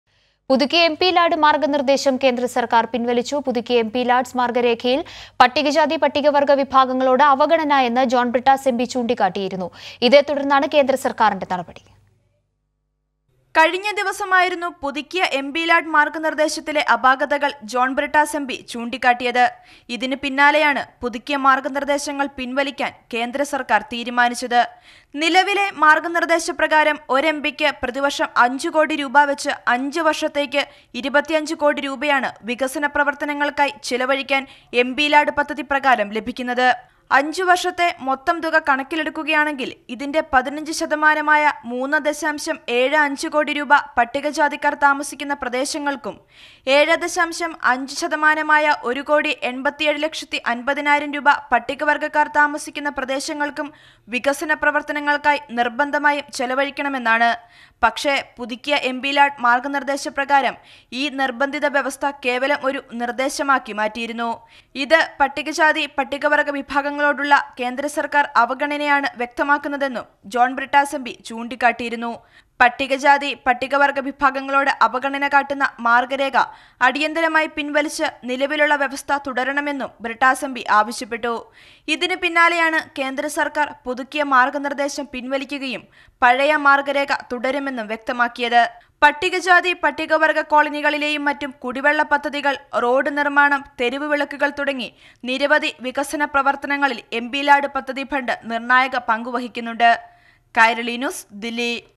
പുതുക്കിയ എംപിലാഡ്‌ മാർഗ്ഗനിർദ്ദേശങ്ങൾ കേന്ദ്രസർക്കാർ പിൻവലിച്ചു | MPLADS | John Brittas MP
മാർഗനിർദേശസങ്ങളിലെ അപാകതകൾ ചൂണ്ടിക്കാണിച്ച് ജോൺ ബ്രിട്ടാസ് എംപി പാർലമെന്റിൽ ചോദ്യങ്ങൾ ഉന്നയിച്ചിരുന്നു.
centre withdraws new MPLADS guidelines